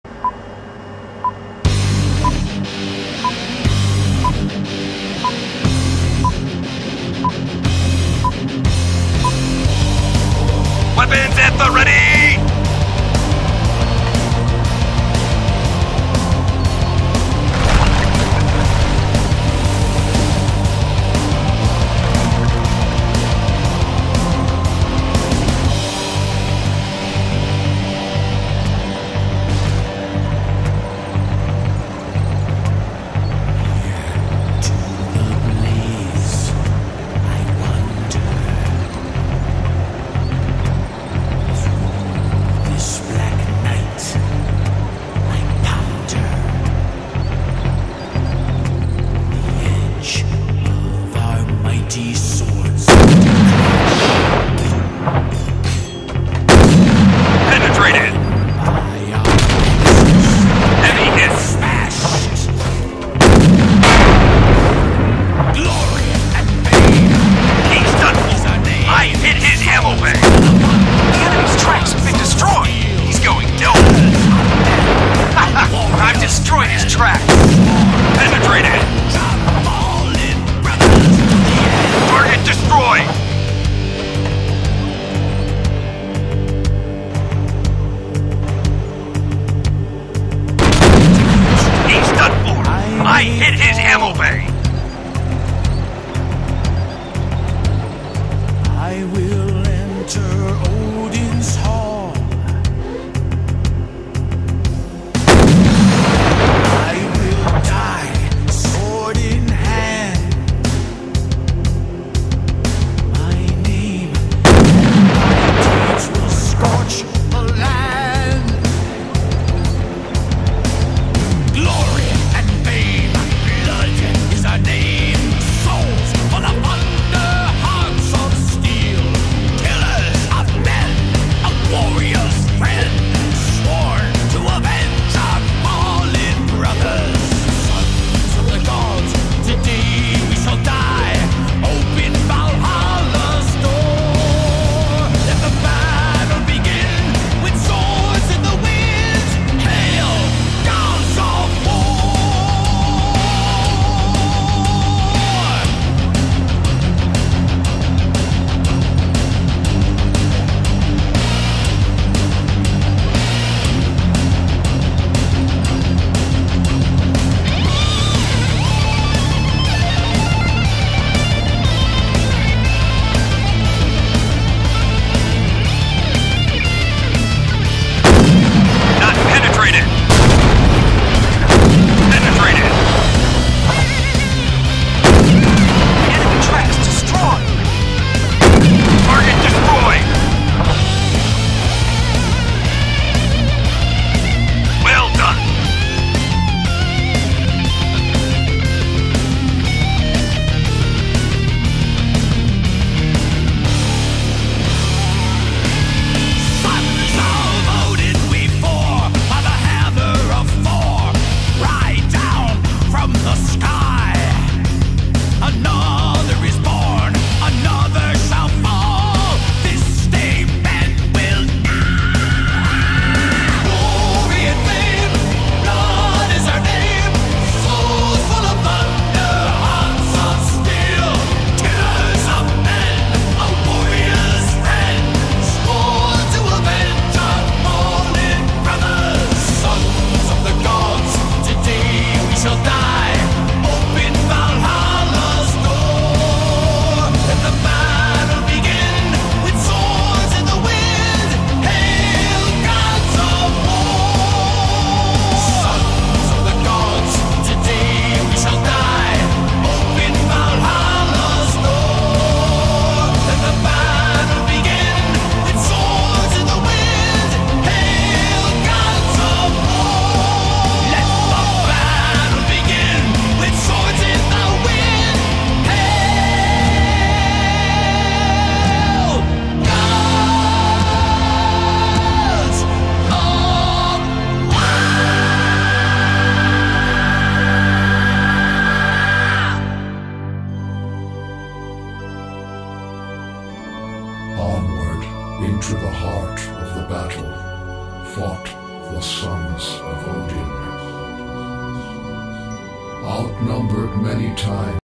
GWT ASU 85 decent battle
After gettin this TD i wanted to add Adv Fiber optics and Adv ammo , but with its stock tracks none could be fitted, so grinding XP with my elite and gold tanks began, since the tracks of this td need 48K xp. but mannnnn its WORTH it XD